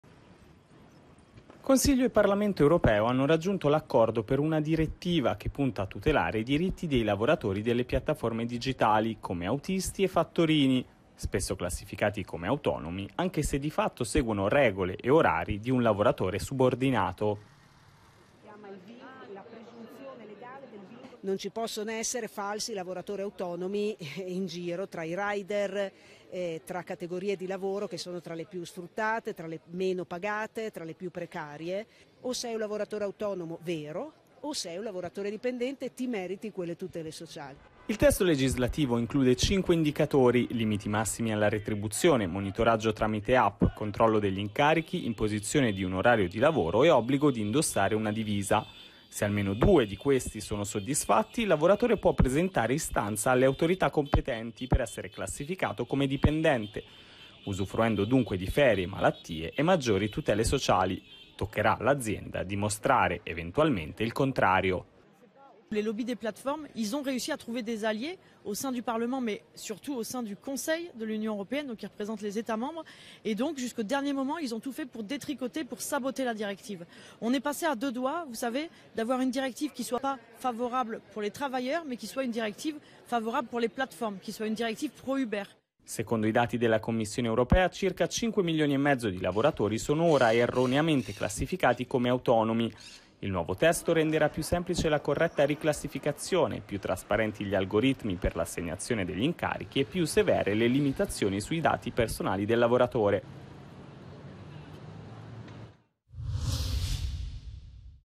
Ue, c'è l'accordo per regolamentare e migliorare le condizioni dei rider
L'accordo per migliorare le condizioni dei lavoratori di piattaforme come Uber o Deliveroo stabilisce quali sono i criteri per considerarli dipendenti